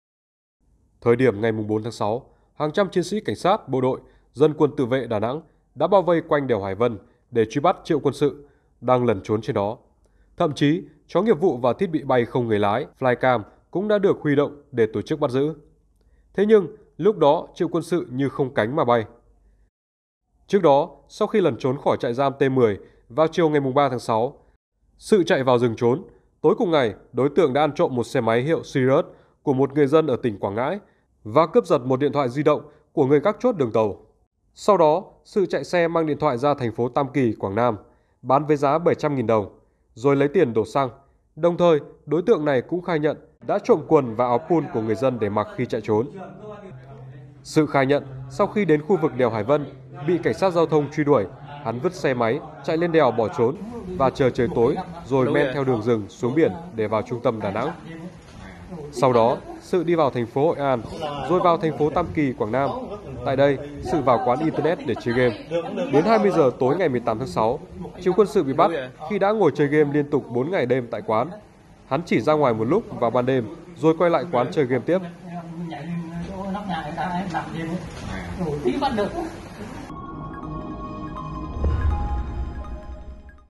Vượt qua vòng vây của hàng trăm bộ đội, công an, Triệu Quân Sự ung dung chơi game trong quán net
Ít ai ngờ, trong vòng vây của hàng trăm cán bộ chiến sĩ các lực lượng, huy động cả chó nghiệp vụ và các phương tiện hiện đại như flycam bao vây quanh đèo Hải Vân, thế nhưng Triệu Quân Sự đã tài tình trốn thoát, ngược theo đường ven biển về lại thành phố Tam Kỳ, chơi game suốt 4 ngày đêm trước khi bị bắt lại.